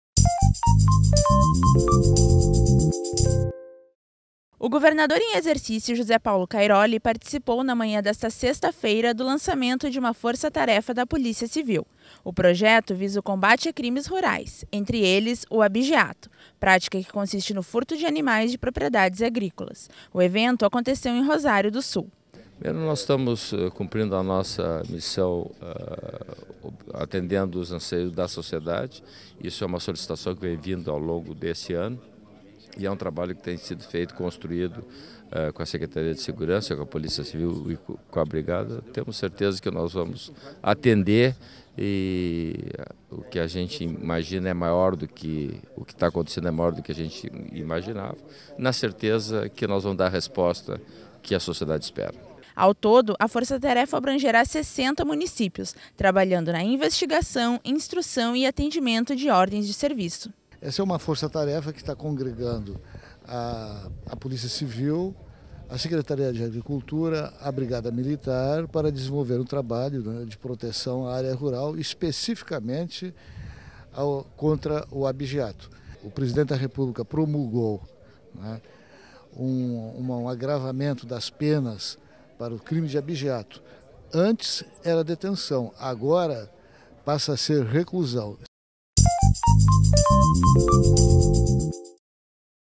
Força-tarefa de combate ao abigeato é lançada em Rosário do Sul
Uma força-tarefa da Polícia Civil (PC) para combater crimes rurais, entre eles o abigeato (furto de animais de propriedades agrícolas) foi lançada, na manhã desta sexta-feira (19), pelo governador em exercício José Paulo Cairoli e o secretário da Segurança Pública Wantuir Jacini, no auditório da Ordem dos Advogados do Brasil (OAB), em Rosário do Sul. Esta ação faz parte da segunda fase do Plano da Segurança Pública, lançado em junho.